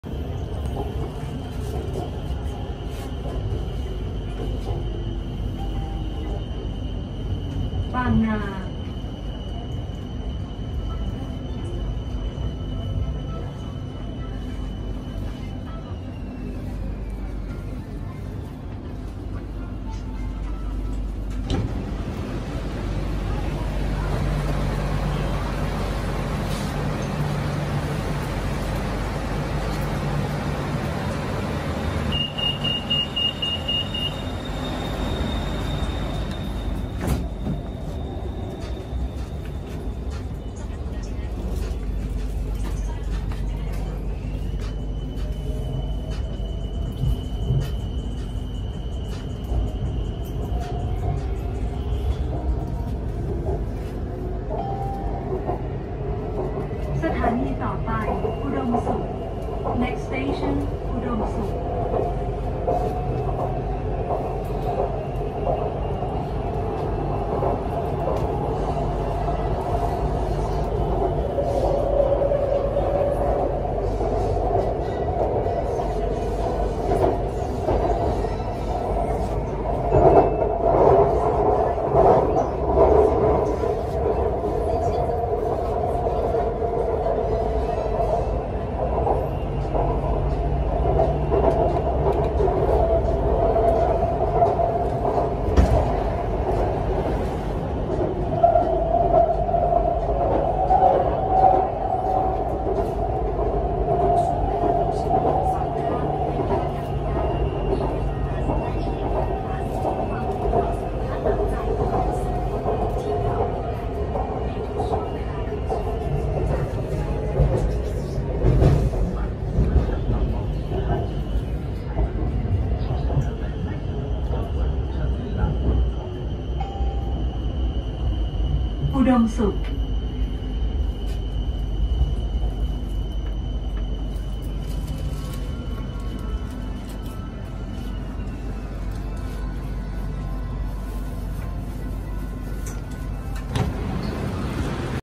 headed to Siam Center Bangkok for malling…from Bearing station to Siam station…
ready for some shopping spree right in the heart of Bangkok…lots and lots to shop and delicious food to eat